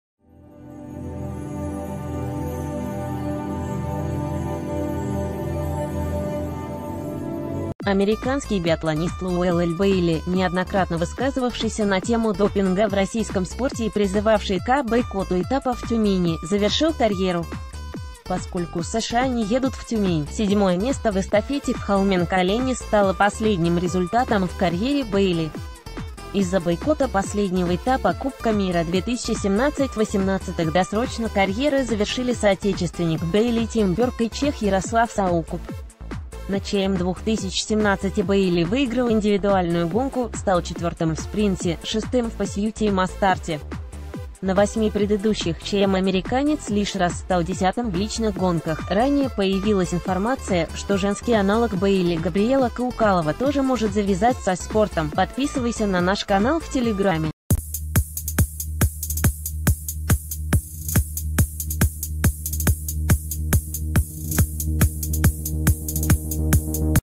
Последние новости | Главный критик российского спорта Лоуэлл Бэйли завершил карьеру
Американский биатлонист Лоуэлл Бэйли, неоднократно высказывавшийся на тему допинга в российском спорте и призывавший к бойкоту этапа в Тюмени, завершил карьеру. Поскольку США не едут в Тюмень, седьмое место в эстафете в Холменколлене стало последним результатом в карьере Бэйли. Из-за бойкота последнего этапа Кубка мира-2017/18 досрочно карьеры завершили соотечественник Бэйли Тим Бёрк и чех Ярослав Соукуп. На ЧМ-2017 Бэйли выиграл индивидуальную гонку, стал четвертым в спринте, шестым в пасьюте и...